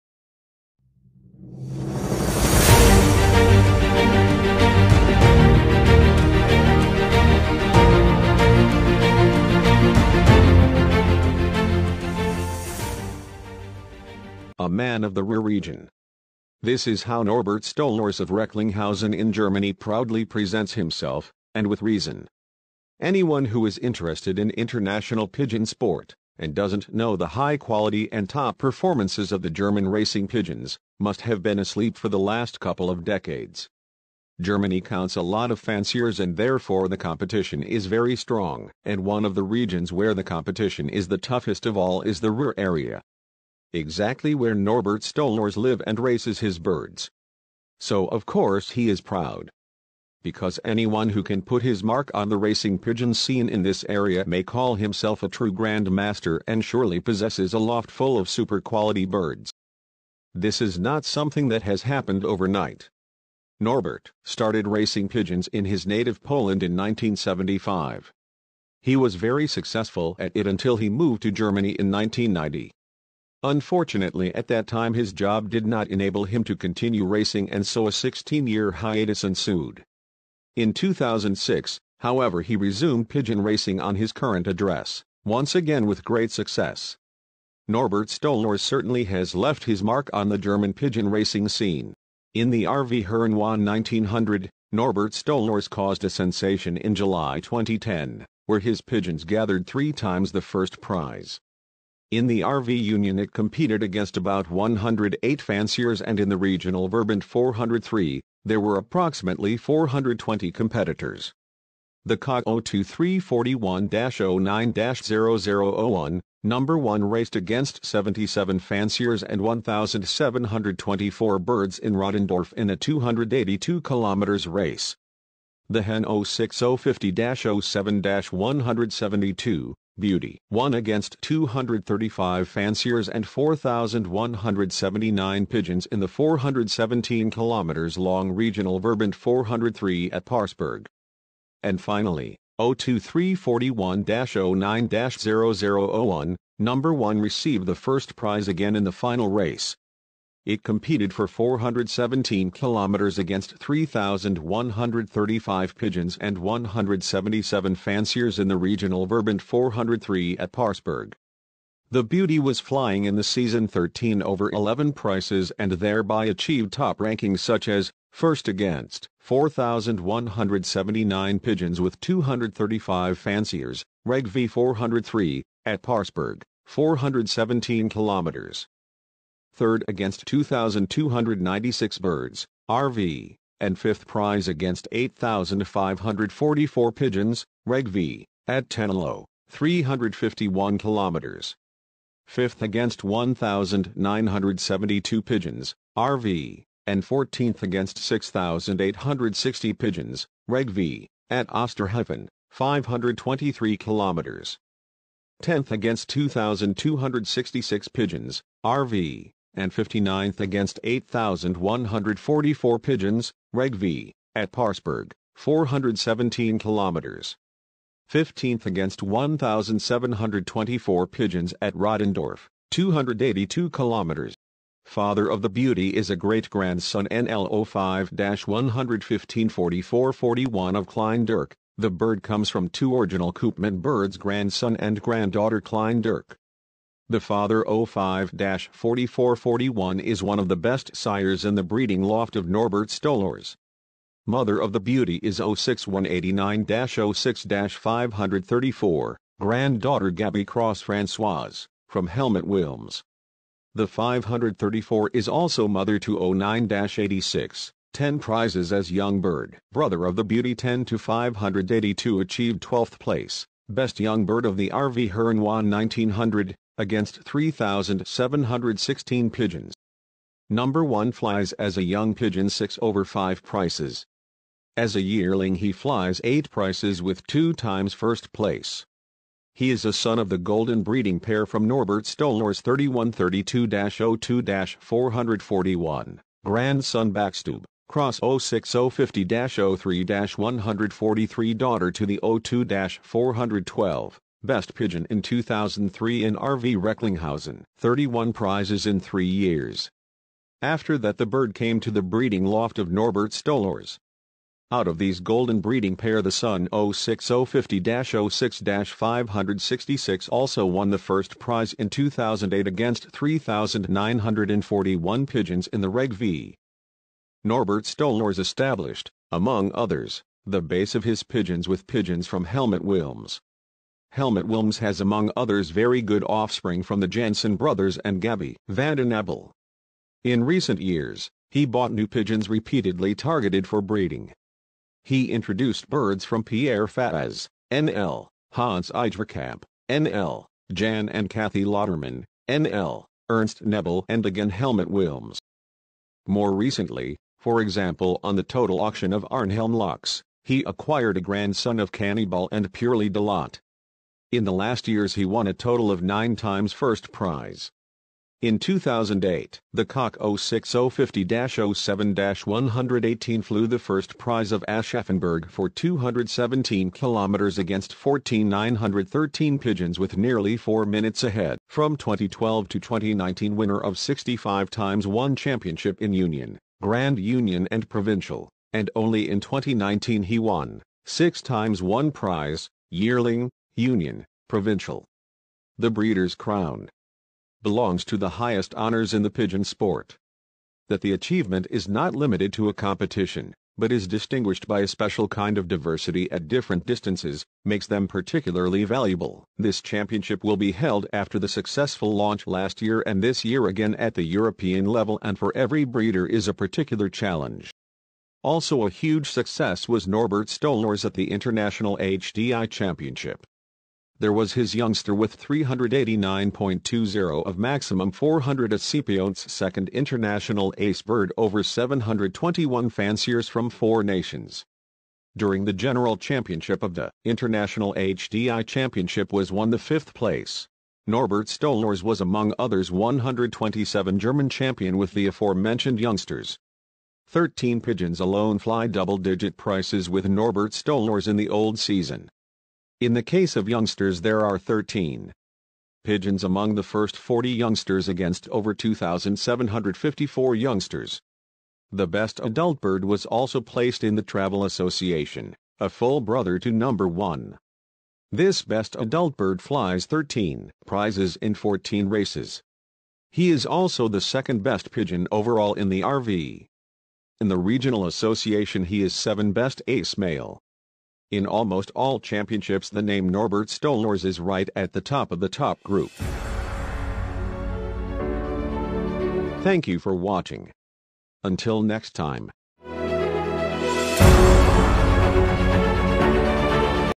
One of the Best Pigeon Fancier from Germany - NORBERT STOLORZ
● Tips
● Method
● Medication
● Motivation
● Feeding

Norbert Stolorz.
A man of the Ruhr region
Successful Pigeon fancier on the south east direction!

Norbert Stolorz lives in Recklinghausen, Germany. This region in the Ruhr area is well known for pigeon racing, here it is one of the biggest rivalries in the pigeon sport in Germany. Norbert Stolorz tears in the RV Herne-Wanne 1900. This RV was in the last years as the largest RV-Union in Germany! Attached is the RV Herne-Wanne 1900 for regional associations 403 Bochum.
Travel direction is the hard South-East direction. The  played  to the total widowhood.

One of the most successful years was certainly the year 2011, it was also one of the toughest years for the pigeons. High temperatures and strong winds have not made easy for pigeons.

This year took place in the Ruhr(National) other 2 flights. For many, the highlight of pigeon breeders throughout the year. With these two wins flights Norbert Stolorz following top positions:

1.Ruhr region Race 12/06/2011 430 KM from Hemau

1.prize - 1801 birds and 82 fanciers RV Herne-Wanne 1900
2.prize - 7114 birds and 328 fanciers Regional Association 403
6.prize - 53 996 birds and 2321 fanciers Ruhr region race.

26/06/2011 2.Ruhr region race from Osterhofen 523 KM

2.prize - 1566 birds and 77 fanciers RV Herne-Wanne 1900
7.prize - 5913    birds and 309 fanciers Regional Association 403
33.prize - 34 878 birds and 1760 fanciers Ruhr region race

In this successful year, achieved under the 2 th place in the "Breeders Crown" Allrounder Championship.
For each read the 3 fastest pigeons on regional or national races Osterhofen (523 KM) ; Wels (619 KM) and the two International races Wien(756 KM) and Nickelsdorf (816 KM)!